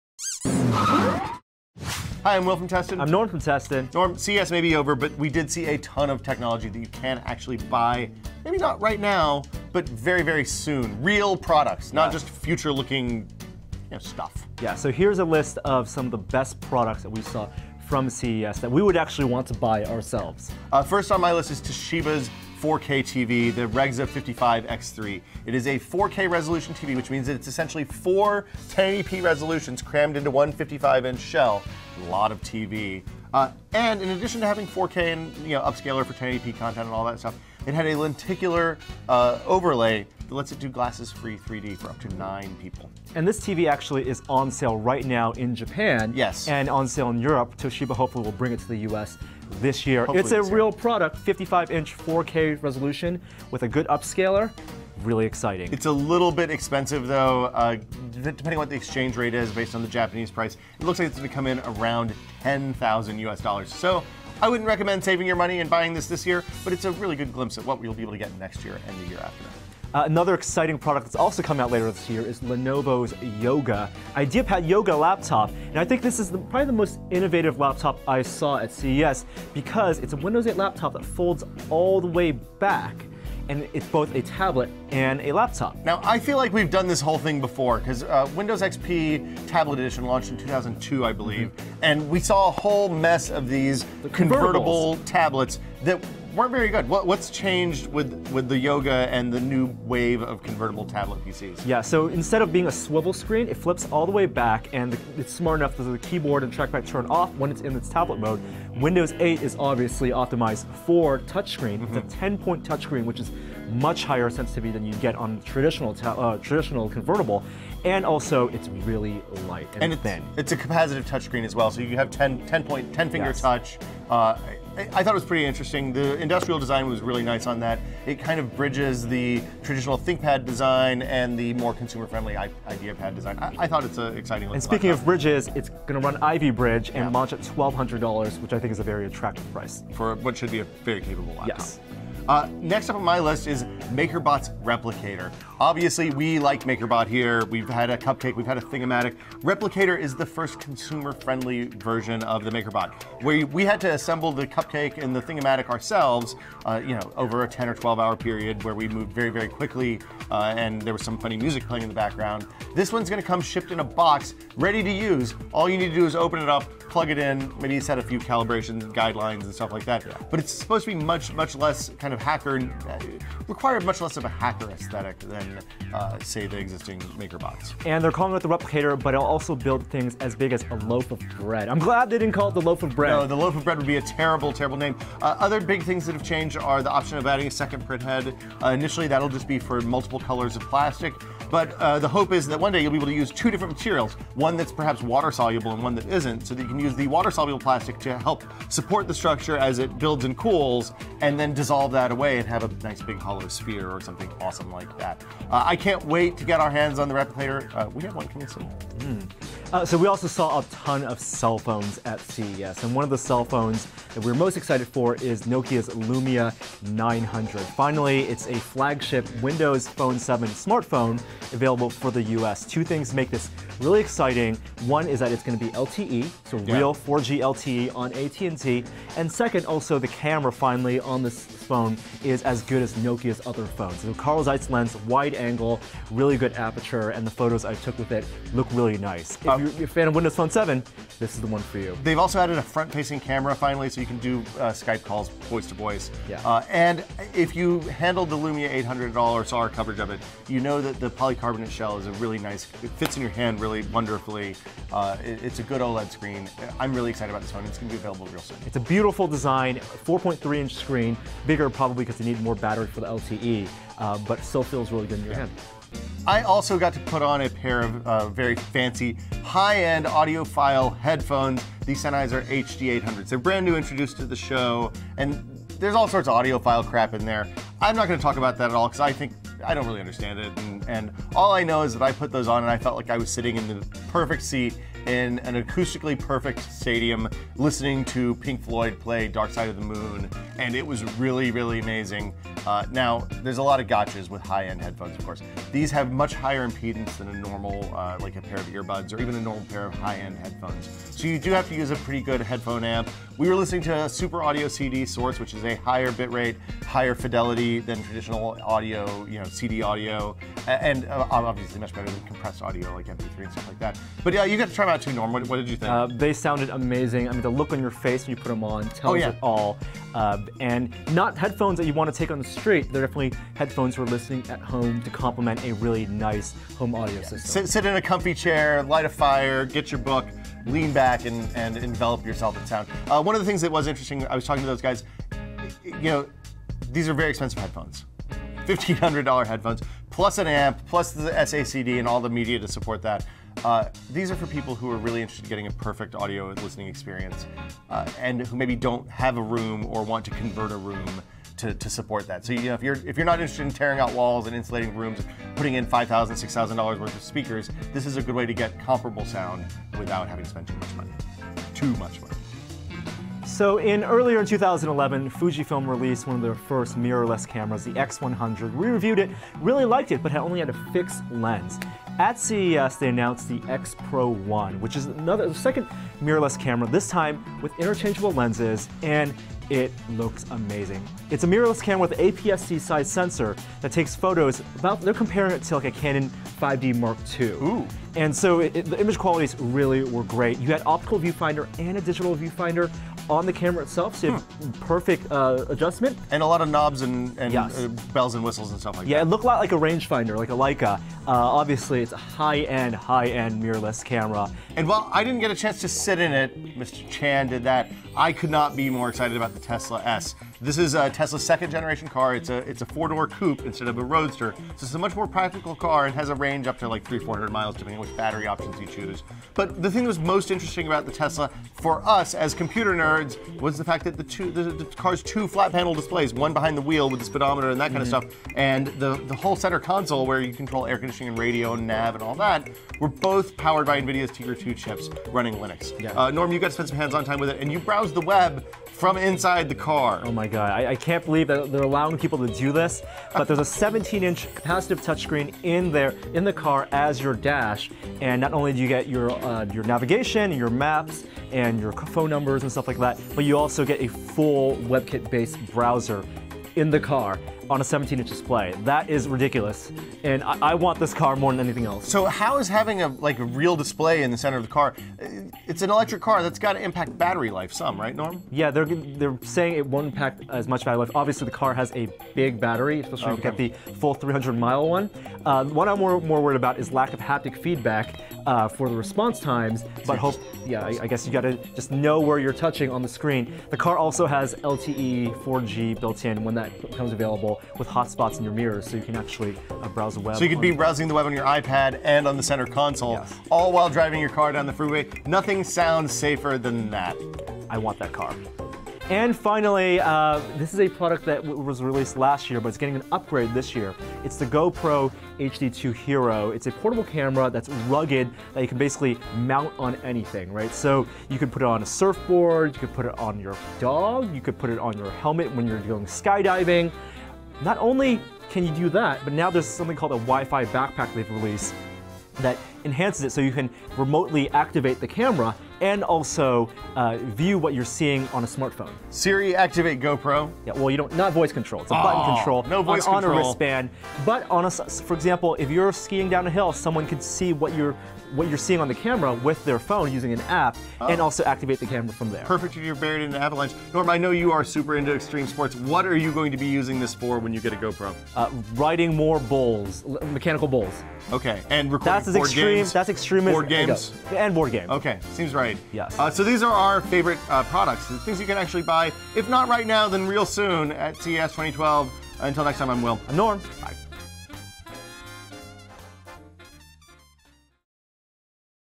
The Best New Products at CES 2012
Most of what you see at CES is years off, but here's the coolest stuff you can actually buy!